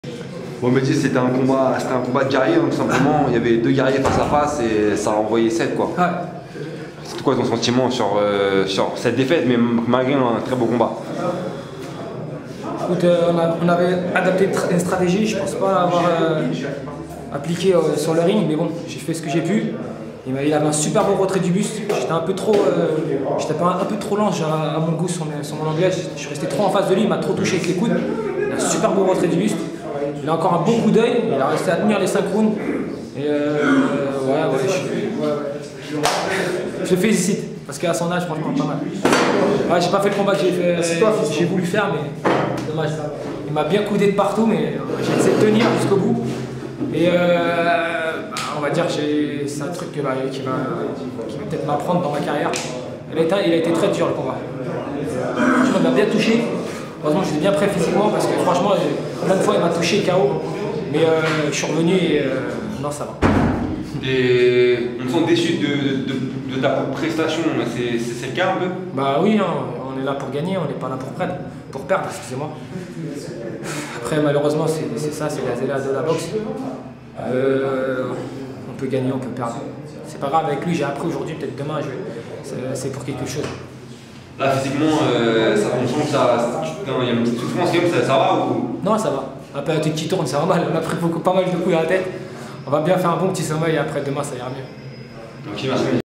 BEST OF SIAM 3 / Mehdi ZATOUT
Réaction après sa défaite aux points face à KAOPON NEK
Jeudi 14 Février 2013
Halle Carpentier